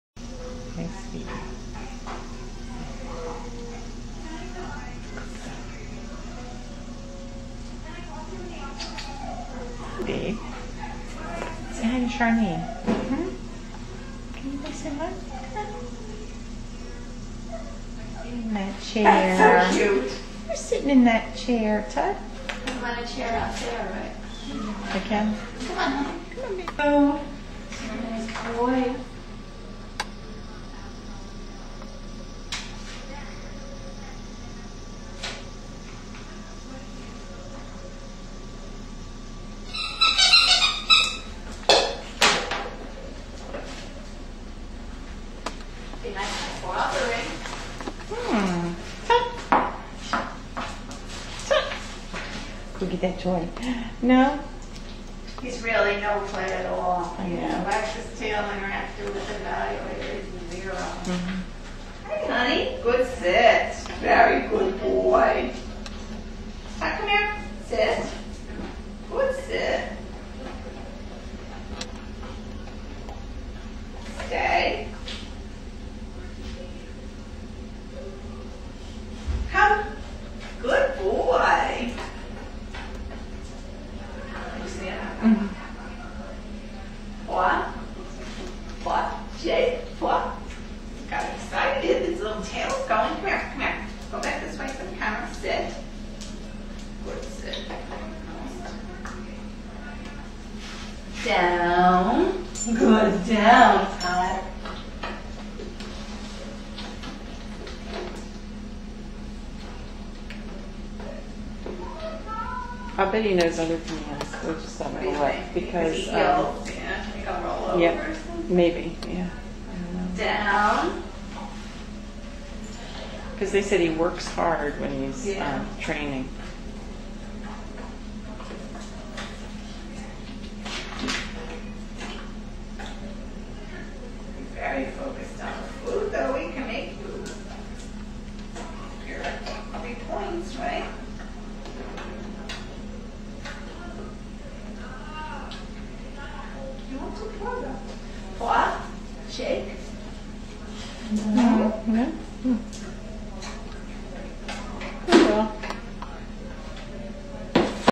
Tut.wmv
Tut is available for adoption at the Monmouth County SPCA.